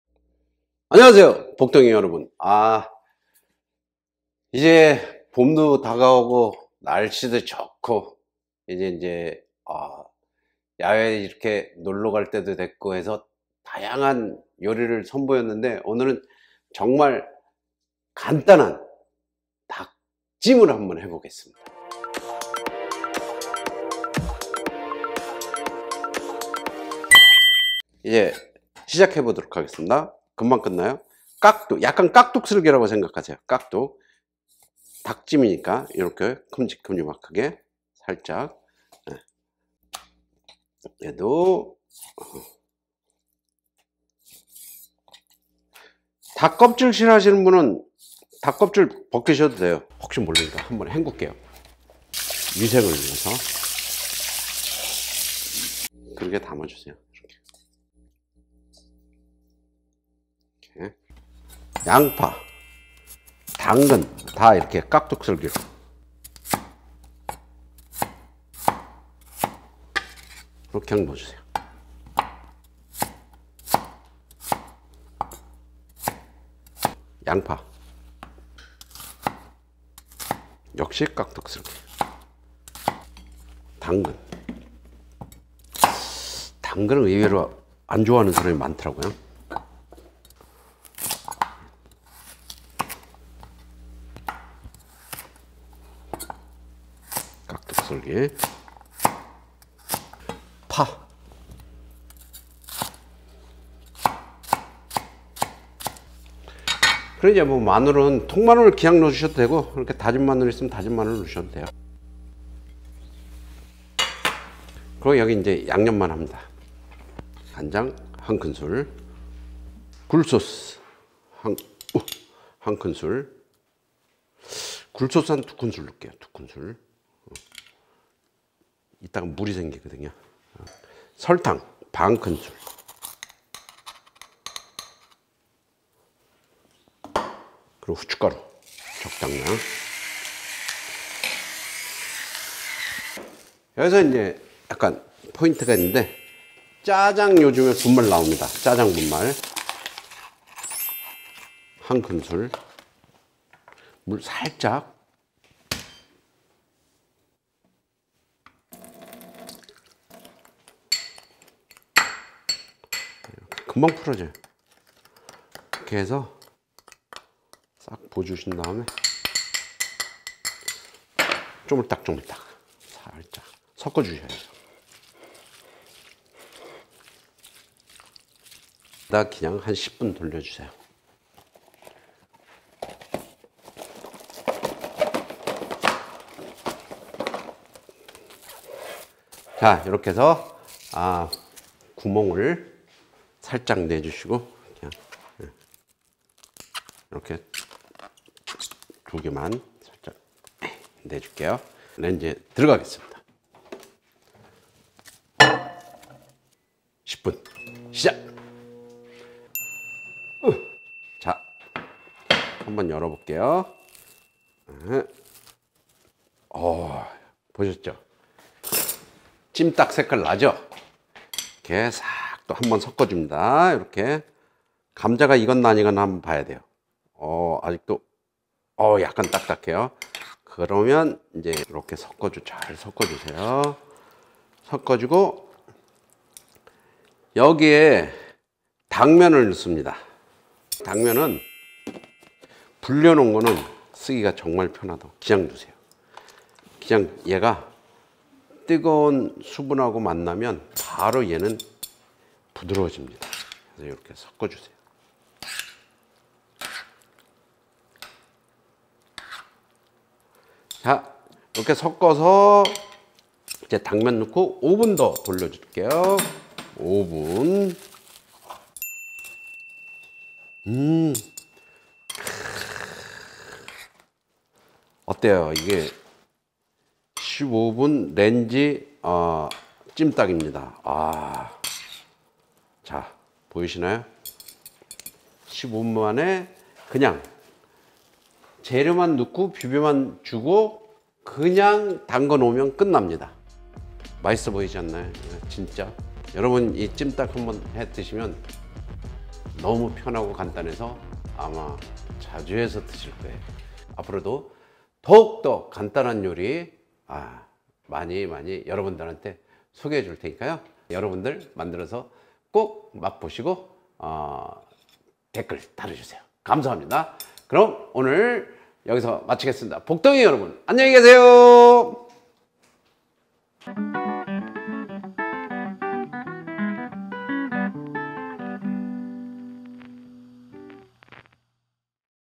이연복의 5분 레시피! x3 찜닭! 전혀 어렵지 않아요!
복덩이 여러분! 만드는데 오래 걸릴 것만 같은 찜닭!
전자레인지를 활용해서 15분만에 만드는 방법을 알려드리겠습니다.
집에서도 쉽고 간편하게 드셔보세요!